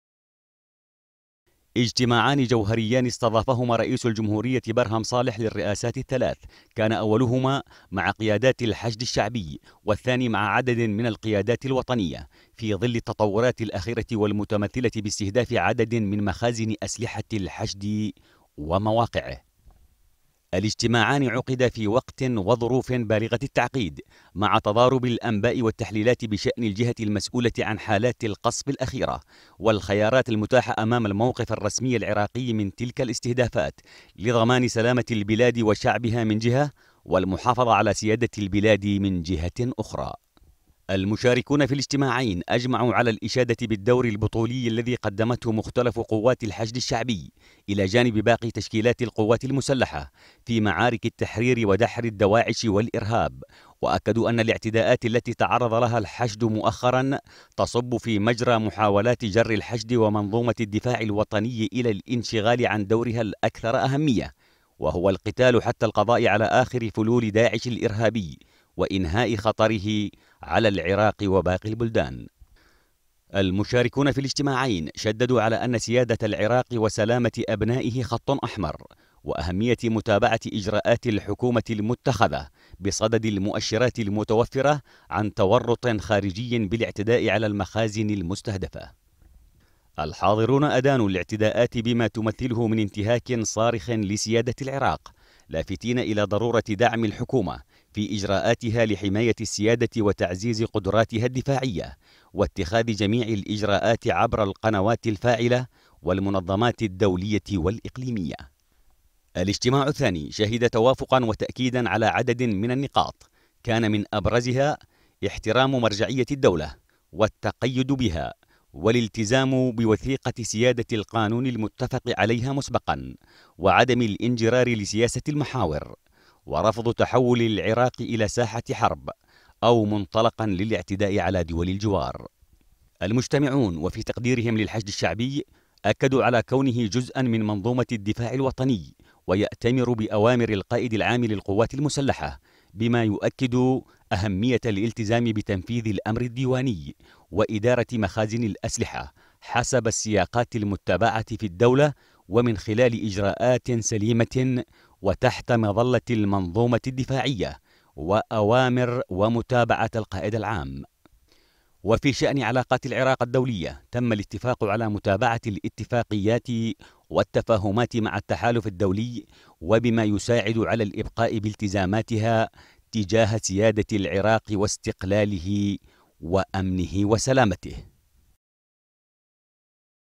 اهم محاور اجتماعي الرئاسات الثلاث مع قيادات الحشد والقادة الوطنيين | تقرير
عقدت الرئاسات الثلاث، اجتماعين منفصلين مع قيادات الحشد الشعبي، وعدد من القيادات الوطنية، لبحث مستجدات الأوضاع، ومناقشة الخيارات للتعامل مع الاعتداءات الأخيرة التي تعرضت لها مواقع ومخازن تابعة للحشد الشعبي.